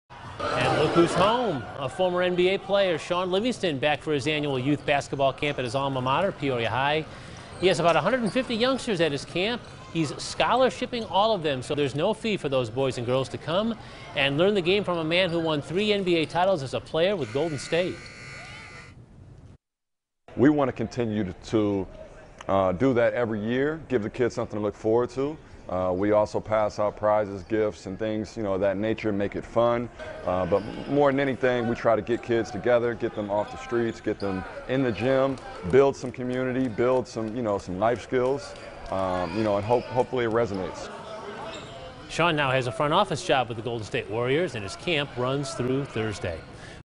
Shaun Livingston camp (6 pm)
Former NBA plyer returns to Peoria for youth basketball camp